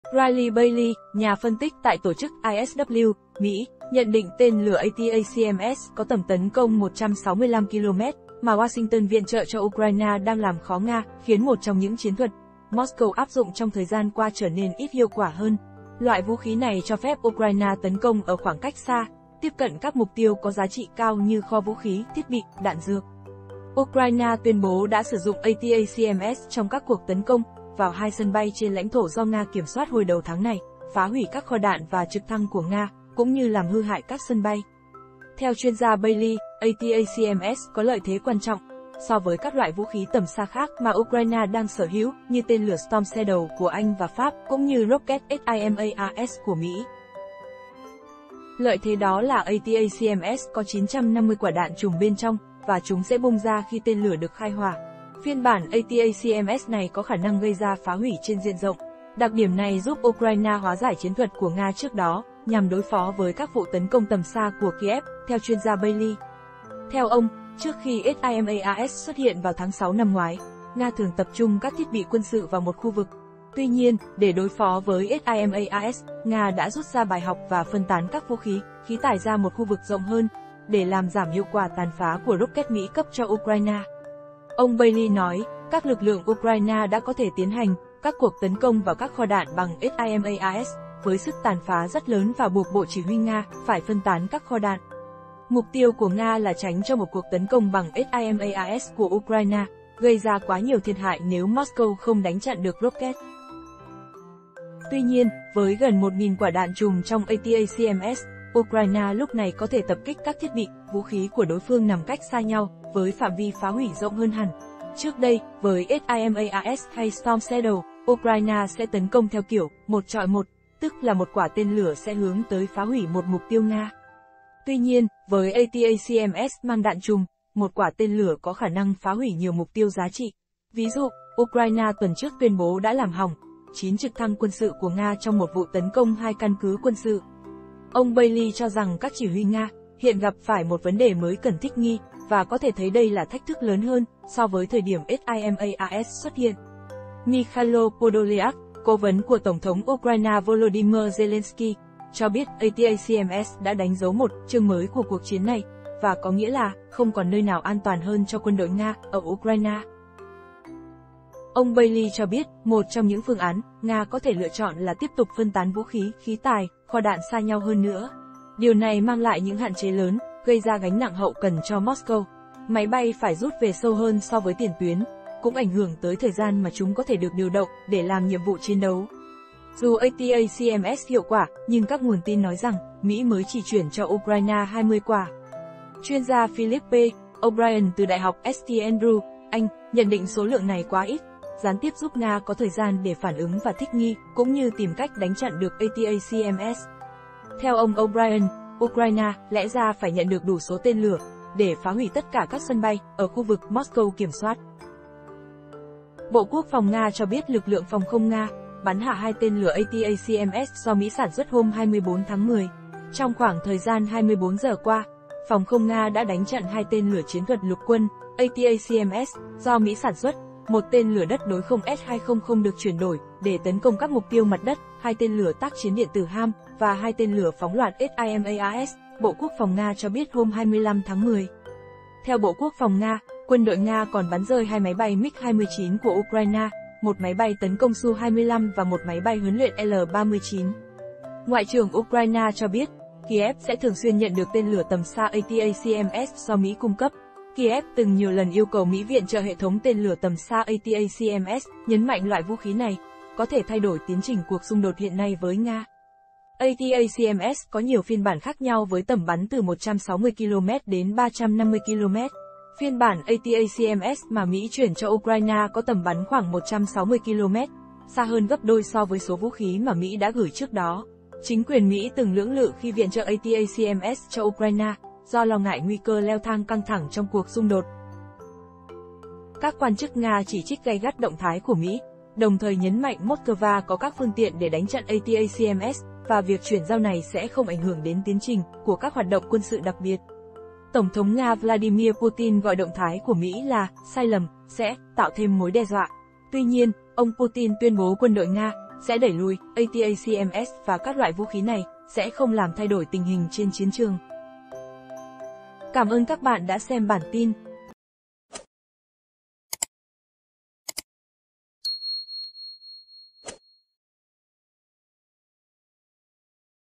VỚI ATACMS, UKRAINE CÓ THỂ KHOÉT SÂU LỖ HỔNG CHIẾN THUẬT CỦA NGA MẶC DÙ NGA TUYÊN BỐ ĐÃ BẮN RƠI 2 TÊ
Riley Bailey, nhà phân tích tại tổ chức ISW (Mỹ), nhận định tên lửa ATACMS có tầm tấn công 165km, mà Washington viện trợ cho Ukraine đang làm khó Nga, khiến một trong những chiến thuật, Moscow áp dụng trong thời gian qua trở nên ít hiệu quả hơn. Loại vũ khí này cho phép Ukraine tấn công ở khoảng cách xa, tiếp cận các mục tiêu có giá trị cao như kho vũ khí, thiết bị, đạn dược.